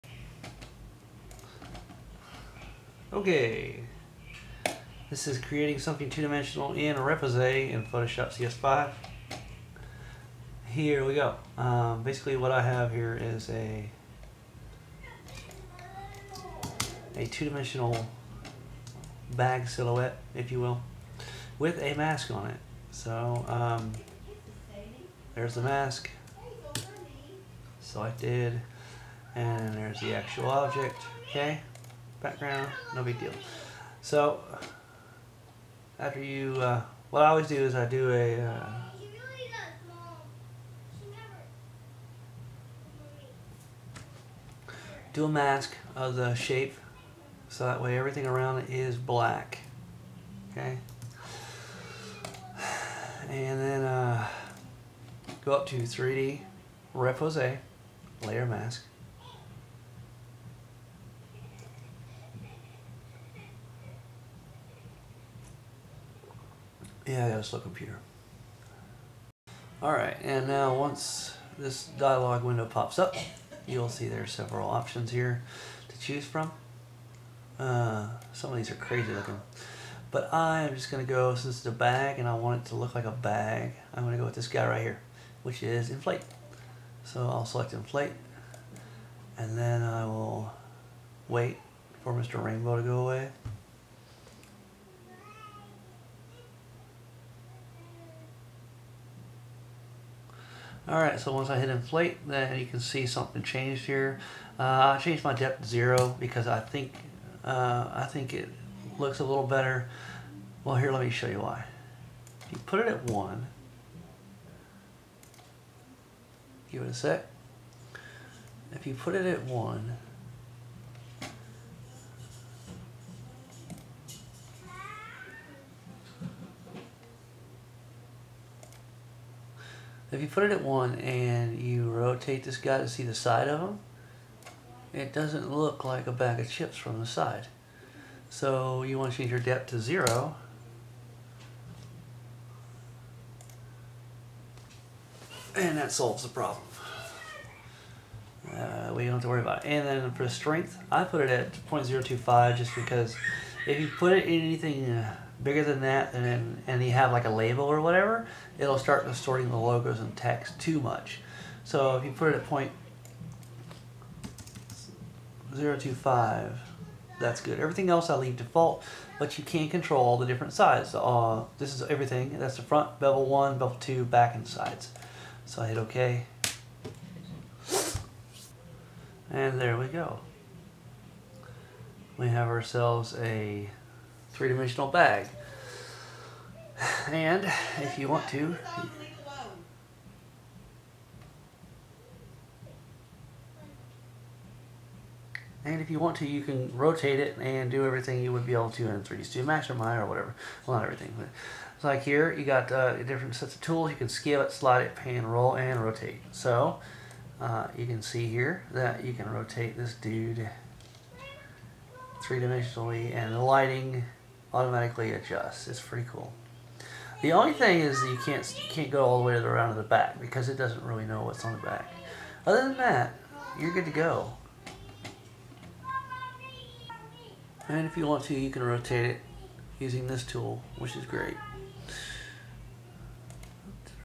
Repousse Tutorial - 3D from 2D
Create a 3 dimensional bag from a flat surface in Photoshop CS5